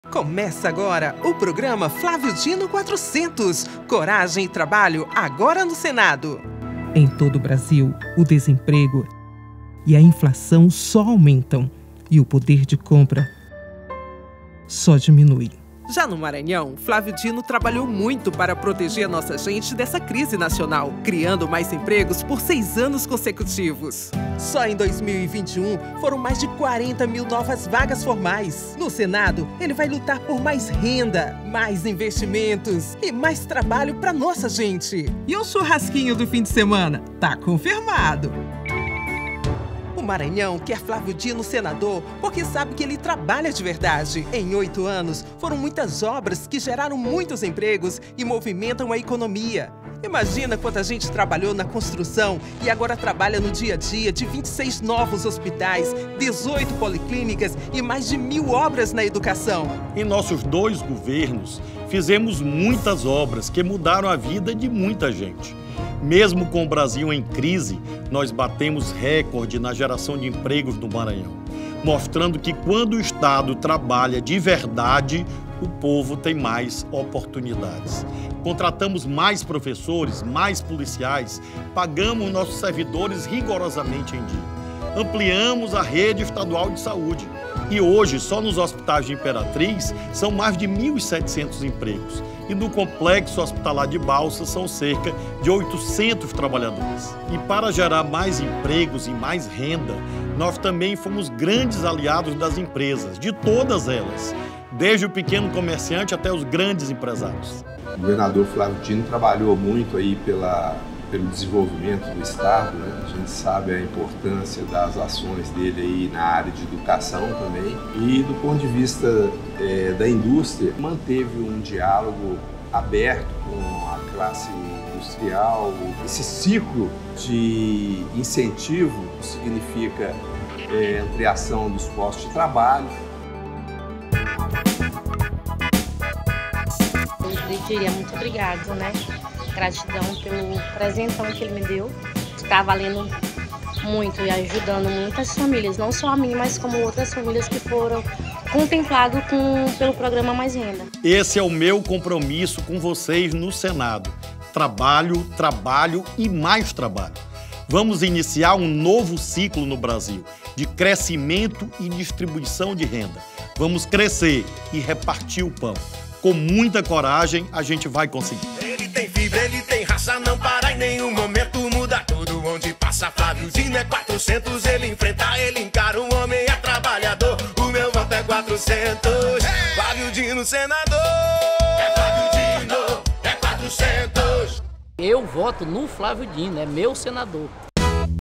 Flávio Dino Senador 400 - Programa Eleitoral 4
O trabalho para a geração de empregos é primordial para um crescimento econômico e, assim, melhorar a vida das famílias.
Pretendo continuar o que fizemos no Governo do Maranhão, agora, no Senado.